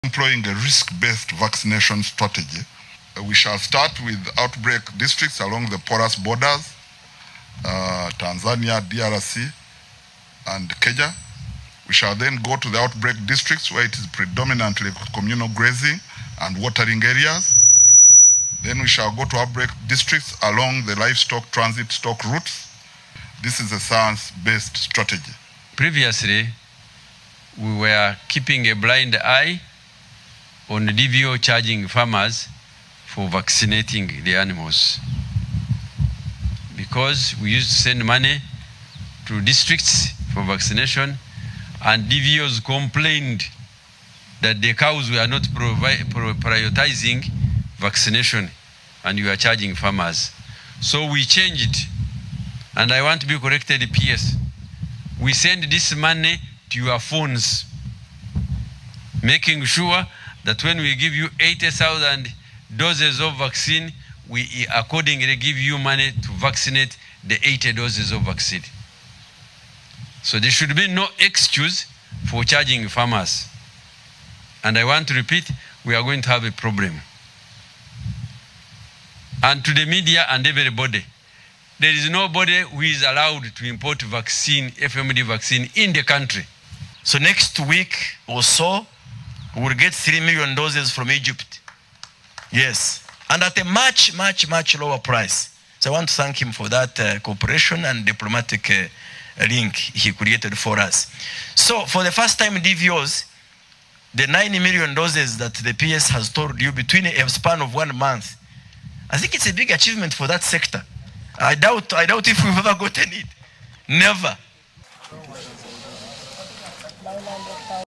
Ministry distributes foot and mouth disease vaccines
The Ministry of Agriculture, Animal Industry, and Fisheries has distributed 900,000 doses of foot and mouth disease vaccines to be used in 46 districts in the cattle corridor affected by the outbreak recorded in November 2023. The ministry had projected it would procure 10 million doses with the 35 billion shillings allocated by the government, having originally asked for 93 billion shillings. As it waits for more of the vaccine, the ministry says it will pay 200 shillings to the veterinary officers for each dose administered, and the farmers will not bear any costs.